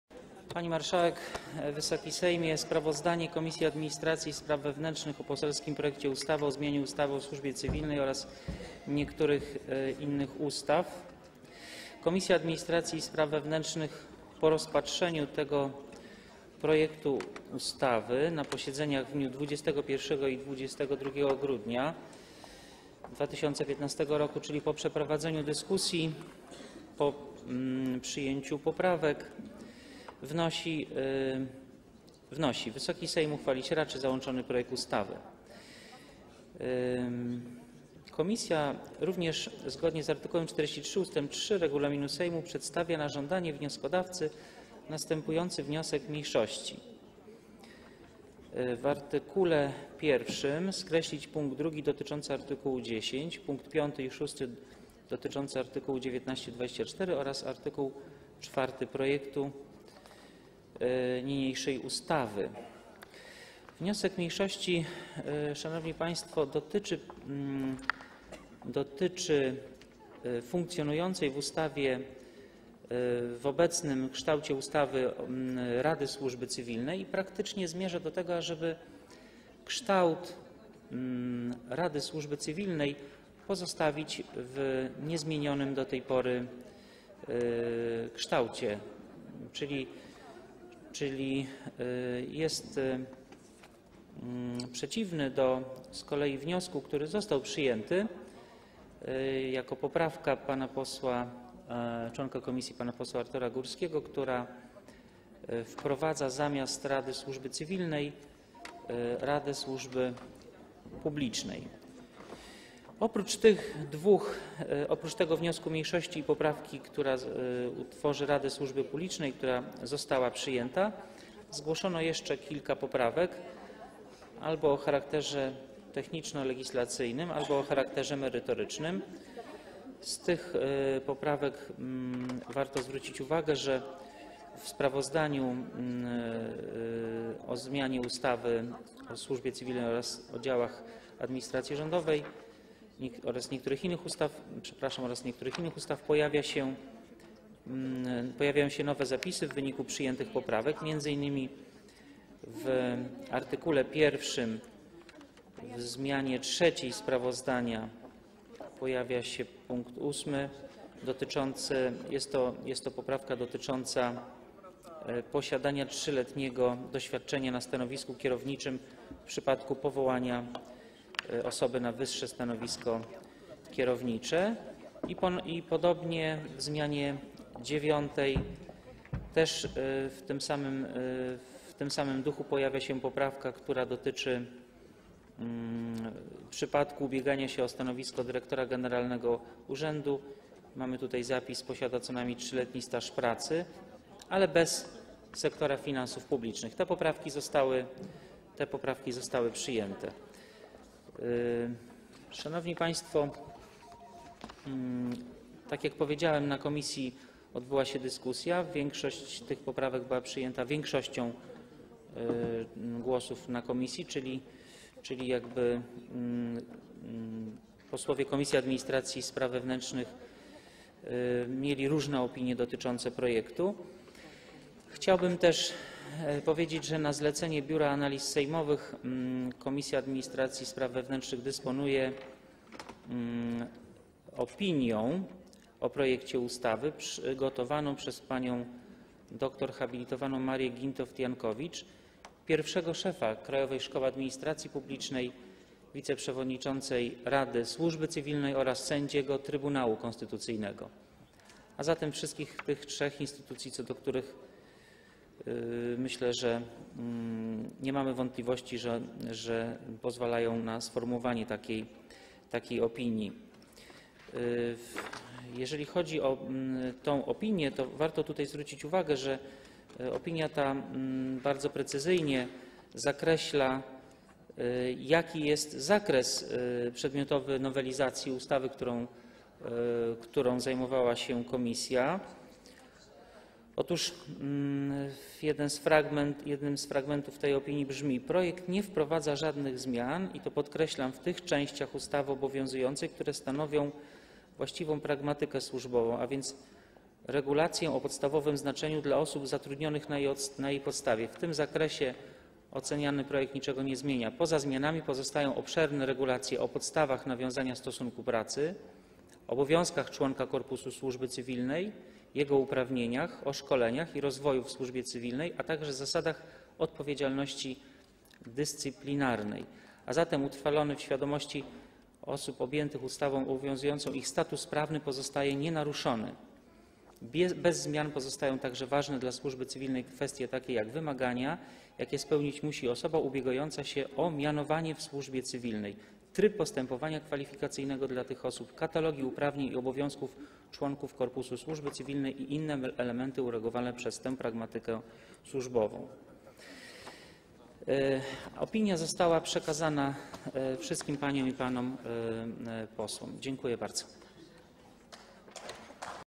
Arkadiusz Czartoryski (PiS) na temat zmian w ustawie o Służbie Cywilnej
7. posiedzenie Sejmu VIII kadencji - 29/12/2015
Sprawozdanie Komisji Administracji i Spraw Wewnętrznych o poselskim projekcie ustawy o zmianie ustawy o służbie cywilnej oraz niektórych innych ustaw (druki nr 119 i 143).

Poseł Sprawozdawca Arkadiusz Czartoryski:
    Pani Marszałek! Wysoki Sejmie! Sprawozdanie Komisji Administracji i Spraw Wewnętrznych o poselskim projekcie ustawy o zmianie ustawy o służbie cywilnej oraz niektórych innych ustaw.
    Komisja Administracji i Spraw Wewnętrznych po rozpatrzeniu tego projektu ustawy na posiedzeniach w dniach 21 i 22 grudnia 2015 r., czyli po przeprowadzeniu dyskusji, po przyjęciu poprawek, wnosi: Wysoki Sejm uchwalić raczy załączony projekt ustawy.
    Komisja również, zgodnie z art. 43 ust. 3 regulaminu Sejmu, przedstawia na żądanie wnioskodawcy następujący wniosek mniejszości: w art. 1 skreślić pkt. 2 dotyczący art. 10, pkt 5 i 6 dotyczący art. 19-24 oraz art. 4 projektu niniejszej ustawy.
    Wniosek mniejszości, szanowni państwo, dotyczy funkcjonującej w obecnym kształcie ustawy Rady Służby Cywilnej i praktycznie zmierza do tego, ażeby Radę Służby Cywilnej pozostawić w niezmienionym do tej pory kształcie, czyli jest przeciwny z kolei do wniosku, który został przyjęty jako poprawka członka komisji pana posła Artura Górskiego, która wprowadza zamiast Rady Służby Cywilnej Radę Służby Publicznej.
    Oprócz tych dwóch: wniosku mniejszości i poprawki, która tworzy Radę Służby Publicznej, która została przyjęta, zgłoszono jeszcze kilka poprawek albo o charakterze techniczno-legislacyjnym, albo o charakterze merytorycznym. Jeżeli chodzi o te poprawki, warto zwrócić uwagę, że w sprawozdaniu o projekcie ustawy o zmianie ustawy o służbie cywilnej oraz niektórych innych ustaw pojawiają się nowe zapisy w wyniku przyjętych poprawek, m.in. w art. 1 zmianie 3. sprawozdania pojawia się pkt 8. Jest to poprawka dotycząca posiadania 3-letniego doświadczenia na stanowisku kierowniczym w przypadku powołania osoby na wyższe stanowisko kierownicze.
    I podobnie w zmianie 9. też w tym samym duchu pojawia się poprawka, która dotyczy przypadku ubiegania się o stanowisko dyrektora generalnego urzędu. Mamy tutaj zapis: posiada co najmniej 3-letni staż pracy, ale bez sektora finansów publicznych. Te poprawki zostały przyjęte.
    Szanowni państwo, tak jak powiedziałem, w komisji odbyła się dyskusja. Większość tych poprawek była przyjęta większością głosów w komisji, czyli posłowie Komisji Administracji i Spraw Wewnętrznych mieli różne opinie dotyczące projektu.
    Chciałbym też powiedzieć, że na zlecenie Biura Analiz Sejmowych Komisja Administracji i Spraw Wewnętrznych dysponuje opinią o projekcie ustawy przygotowaną przez panią dr hab. Marię Gintowt-Jankowicz, pierwszego szefa Krajowej Szkoły Administracji Publicznej, wiceprzewodniczącą Rady Służby Cywilnej oraz sędziego Trybunału Konstytucyjnego, a zatem wszystkich tych trzech instytucji, co do których, jak myślę, nie mamy wątpliwości, że pozwalają na sformułowanie takiej opinii.
    Jeżeli chodzi o tę opinię, to warto tutaj zwrócić uwagę, że opinia ta bardzo precyzyjnie określa, jaki jest zakres przedmiotowy nowelizacji ustawy, którą zajmowała się komisja. Otóż jeden z fragmentów tej opinii brzmi: Projekt nie wprowadza żadnych zmian - i to podkreślam - w tych częściach ustawy obowiązującej, które stanowią właściwą pragmatykę służbową, a więc regulację o podstawowym znaczeniu dla osób zatrudnionych na jej podstawie. W tym zakresie oceniany projekt niczego nie zmienia. Poza zmianami pozostają obszerne regulacje o podstawach nawiązania stosunku pracy, obowiązkach członka korpusu służby cywilnej, jego uprawnieniach, o szkoleniach i rozwoju w służbie cywilnej, a także zasadach odpowiedzialności dyscyplinarnej. A zatem utrwalony w świadomości osób objętych ustawą obowiązującą ich status prawny pozostaje nienaruszony. Bez zmian pozostają także ważne dla służby cywilnej kwestie, takie jak: wymagania, jakie spełnić musi osoba ubiegająca się o mianowanie w służbie cywilnej, tryb postępowania kwalifikacyjnego dla tych osób, katalogi uprawnień i obowiązków członków korpusu służby cywilnej i inne elementy uregulowane przez tę pragmatykę służbową.
    Opinia została przekazana wszystkim paniom i panom posłom. Dziękuję bardzo. (Oklaski)